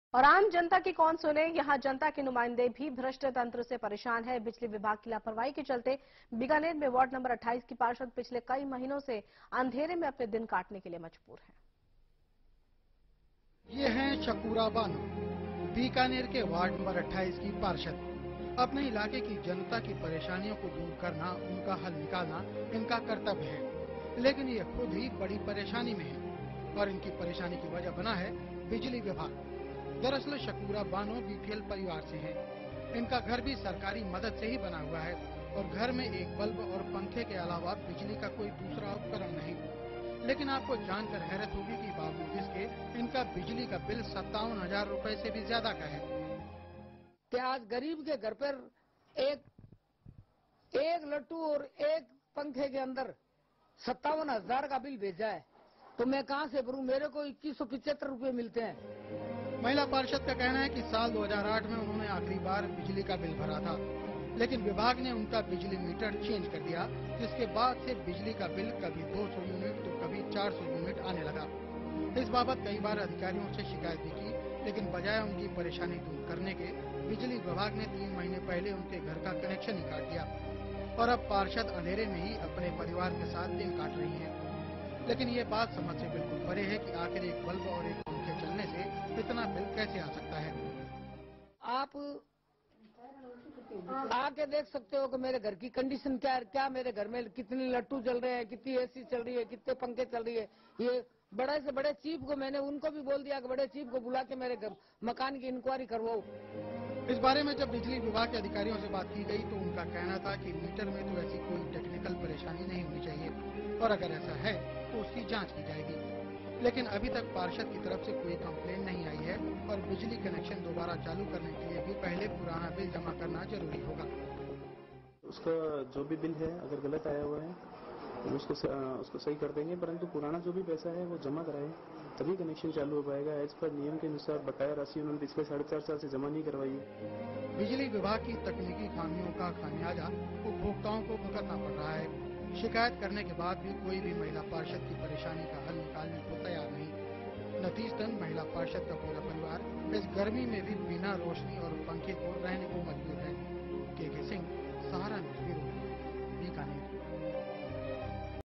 Helpless woman corporator of Bikaner
Bikaner's women corporator is the latest victim of corrupt management. Shakuna Bano has been asked to pay Rs 57000 electricity bill while she uses just a fan and bulb in her house.